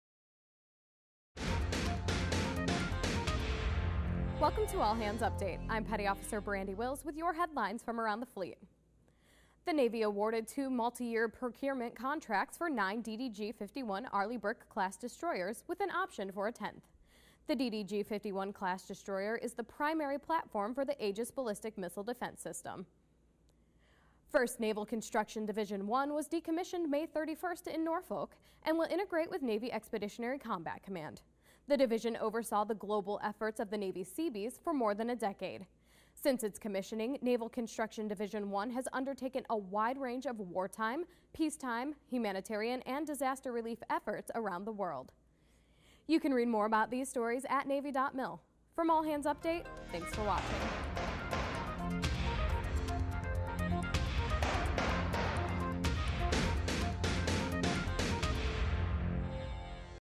DDG 51 Class Ship Multiyear Procurement Construction Contracts Awarded
Headlines for Thursday, June 6, 2013: DDG 51 Class Ship Multiyear Procurement Construction Contracts Awarded; First Naval Construction Division Decommissioned